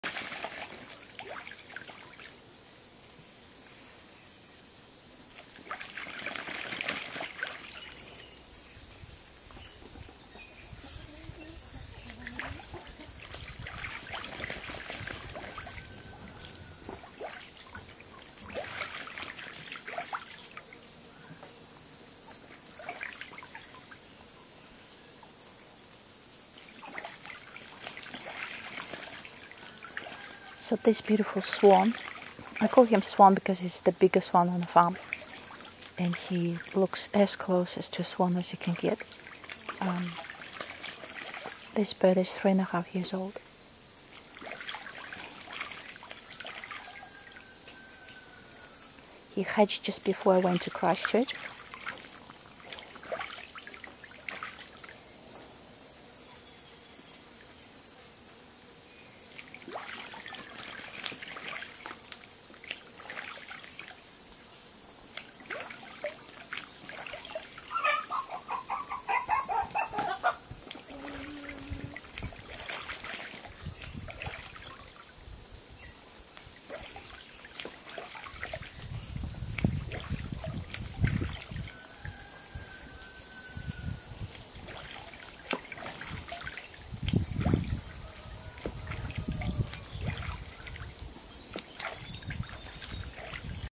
Ducklings Daddy Is Splashing In A Water
Amazing Duck World.  This Drake  is  1.100  Days old  He is so Majestic and Beautiful.